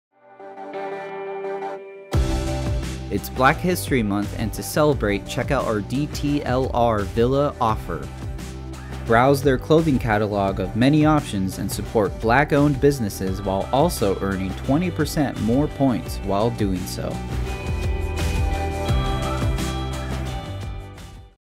Check Out Our DTLR Villa Offer on RewardFish!
With it being Black History Month, check out our DTLR Villa offer! Browse their large catalogue of clothing & support black owned businesses while also earning 20% more points while doing so.